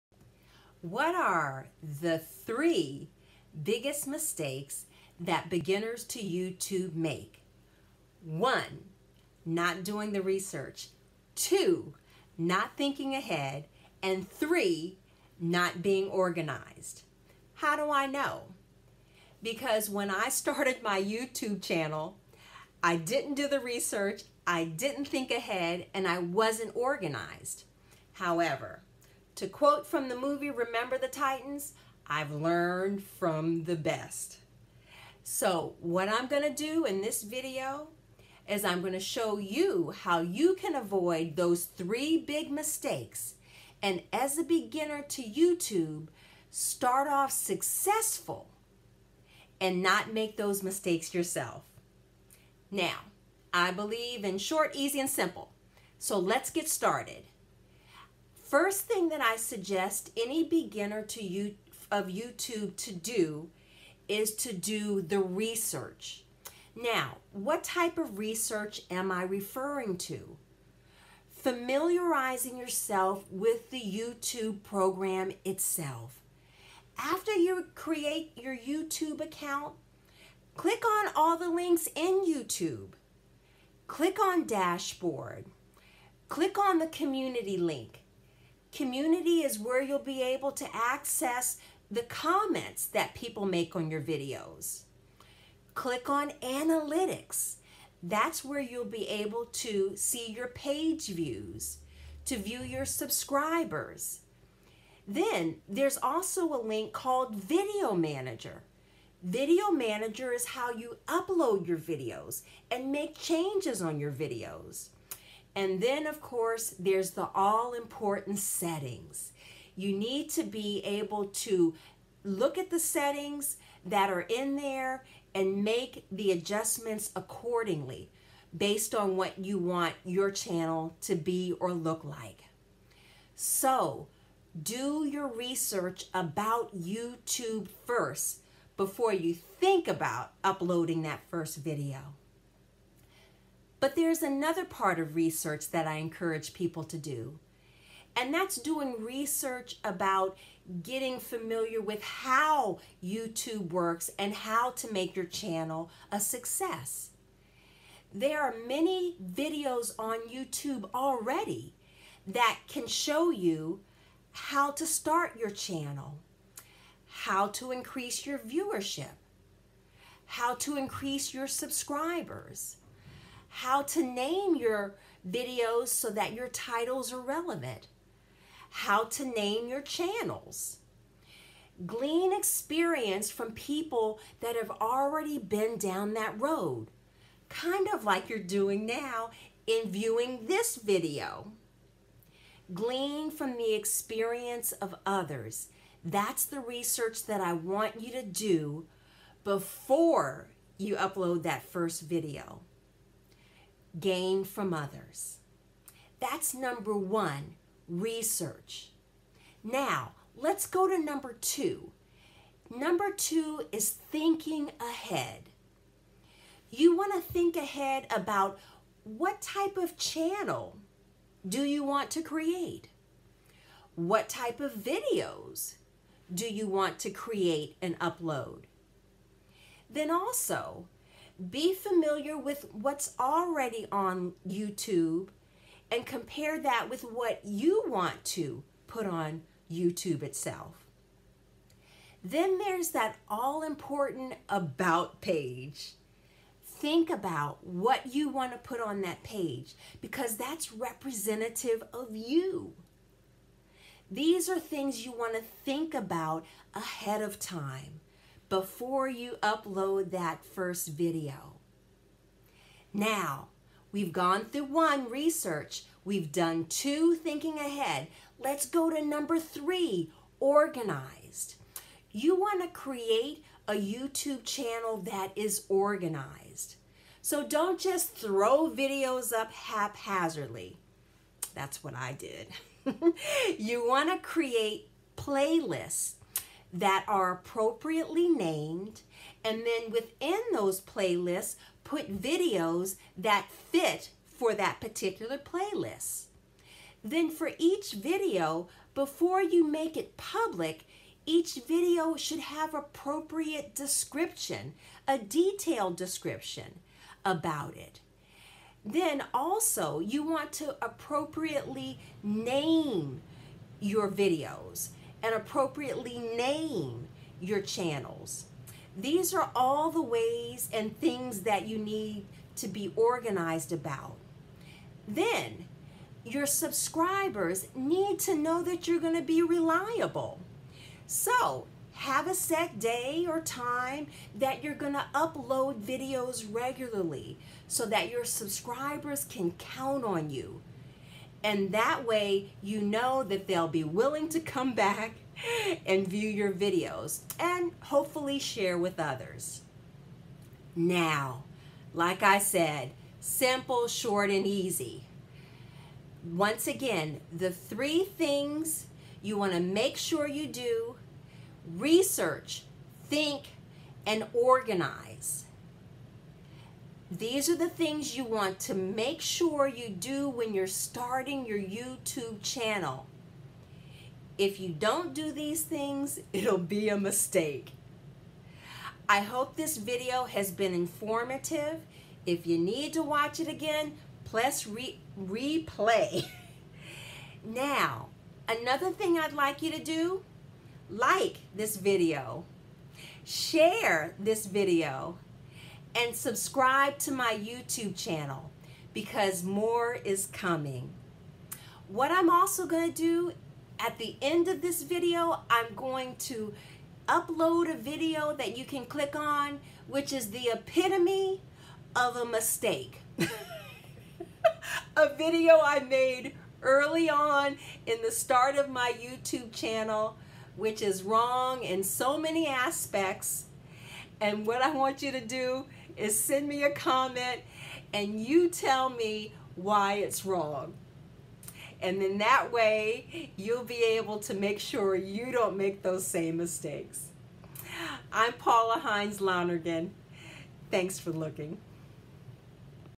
How to Get Started on YouTube and Avoid the 3 Biggest Mistakes
There are 3 (THREE) big mistakes that beginners to YouTube make:

1. Not doing the Research
2. Not Thinking ahead
3. Not being Organized

I know, because I made those mistakes when I first started my YouTube channel. But, I have “learned from the best” as I quote from Remember the Titans

Side note: my daddy is the featured assistant coach in the movie, Yes, a shameless plug, I know.

I started YouTube without doing the research, without thinking, or organizing. It’s better to follow do ALL these steps before you get started.

All beginners to YouTube need to watch my video How to Get Started on YouTube and Avoid the 3 Biggest Mistakes before you upload your first video. So, you, too, can learn from the best!

My video is simple, easy, and short. Please enjoy.

Be sure to send me a comment after you watch.

New videos are uploaded weekly.